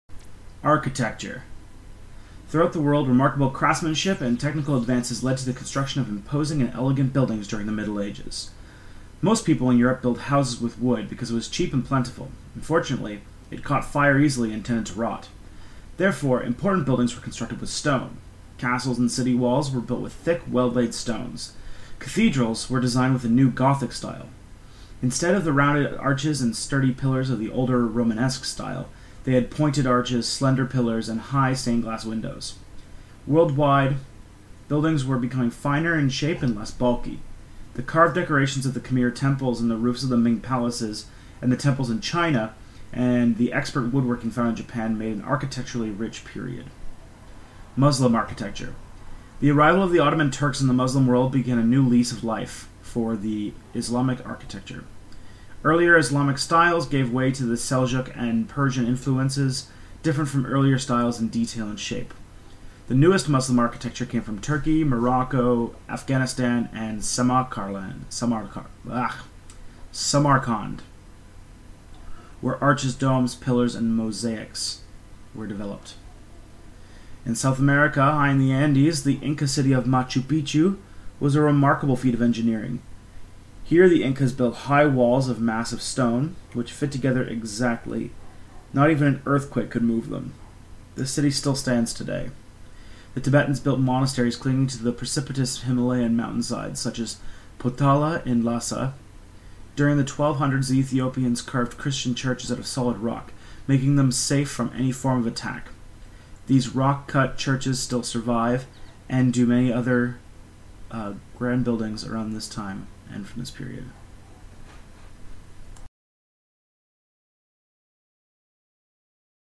The Histories Part 98: Architecture 1101 - 1460
Throughout the world, remarkable craftsmanship and technical advances led to the construction of imposing and elegant buildings during the Middle Ages.